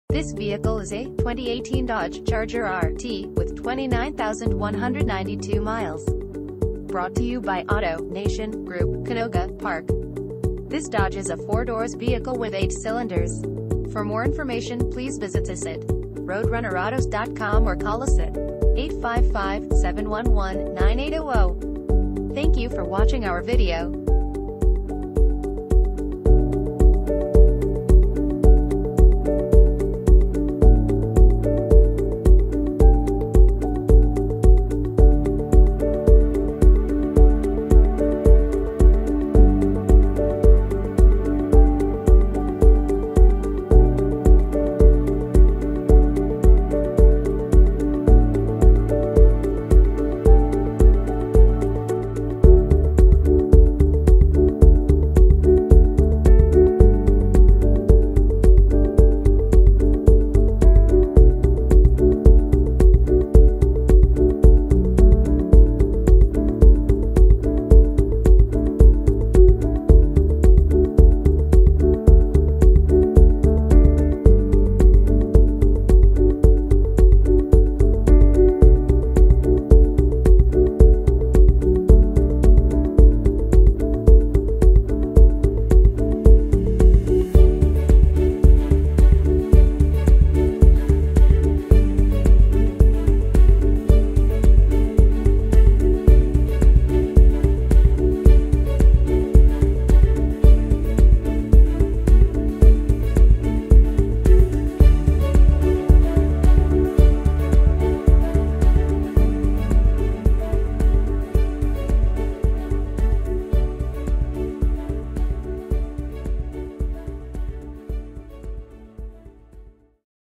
2018 Dodge Charger - Auto Nation Group - Canoga Park
Auto Nation Group - Canoga Park
21024 Sherman Way

2018 Dodge  Charger

Clean title, 3 Months / 3,000 Miles Powertrain, 29,192 miles, Automatic, Black interior, Luxury, RWD: Rear Wheel Drive, 4 doors, 8 cylinders, Gasoline, MPG City 16 and 25 in Hwy.